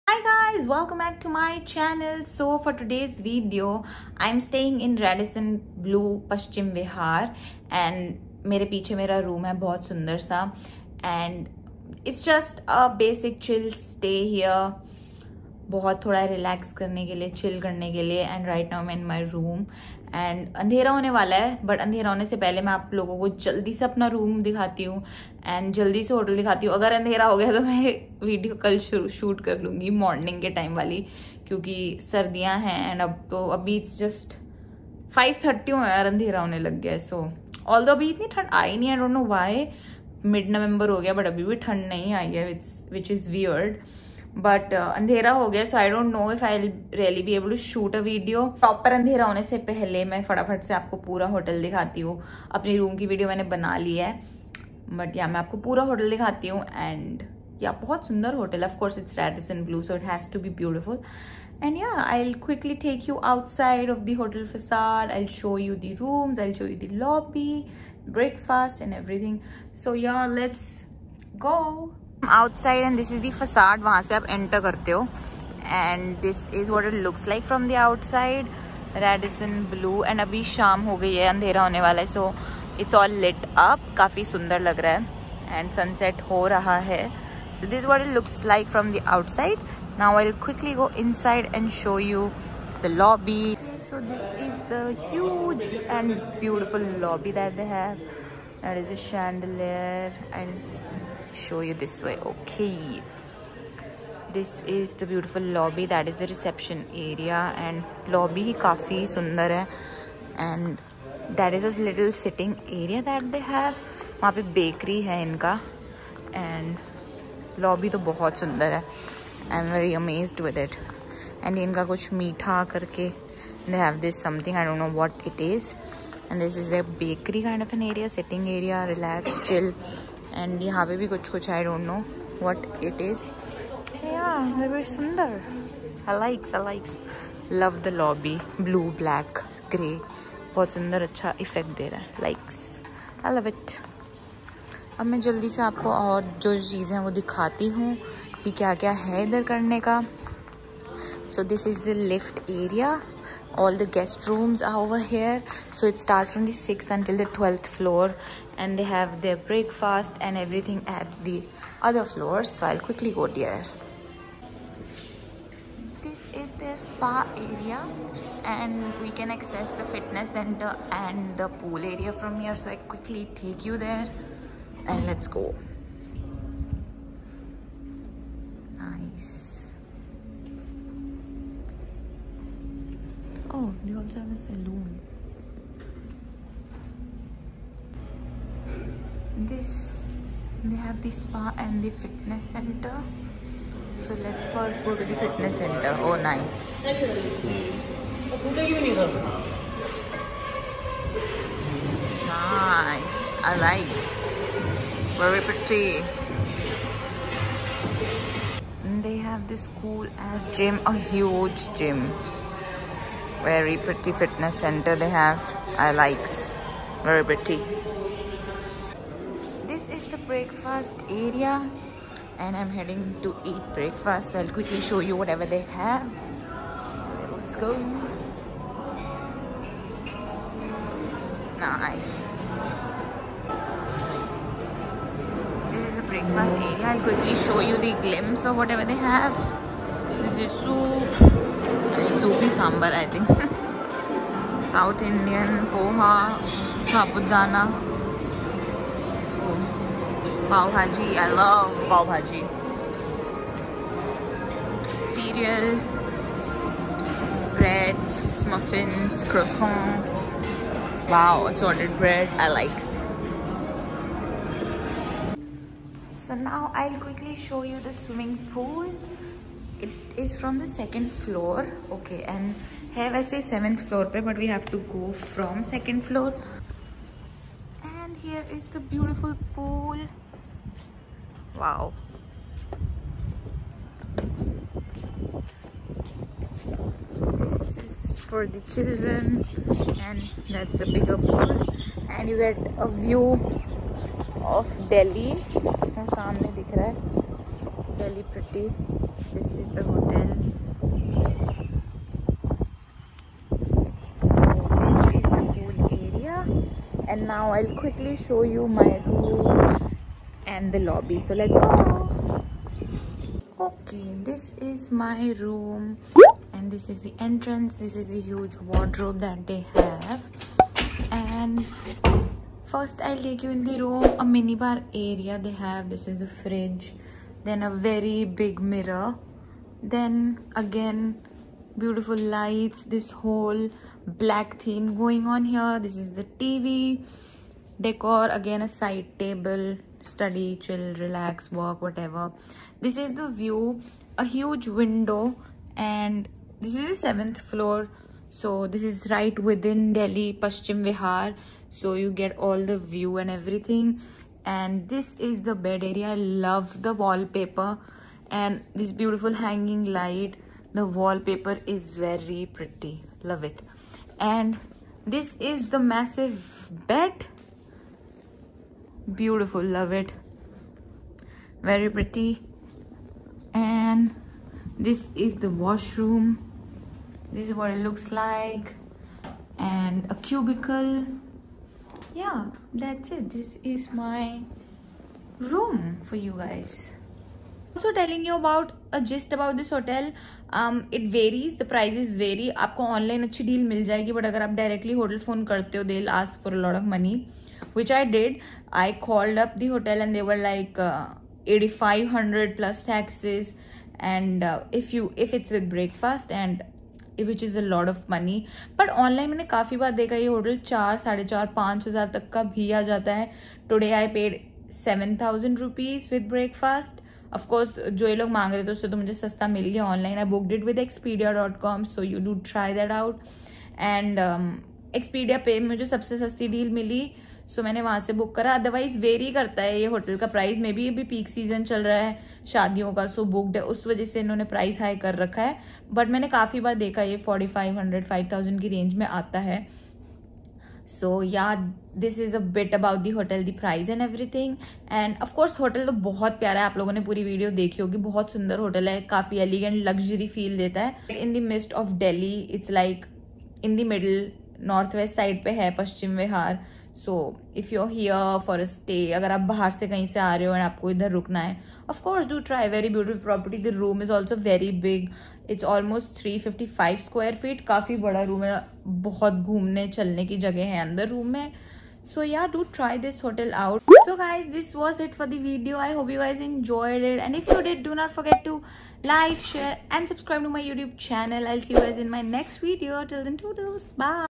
Radisson Blu Paschim Vihar Delhi Hotel Tour | Luxury Hotel in North West Delhi | The Peppy Persona
Hello guys, welcome back to a brand new video on my channel, I stayed with Radisson Blue Paschim Vihar Delhi for a night, and my stay was really very relaxing and soothing, this hotel is right in the middle of the city, and gives you full luxury vibes. A must stay hotel if you are coming to Delhi.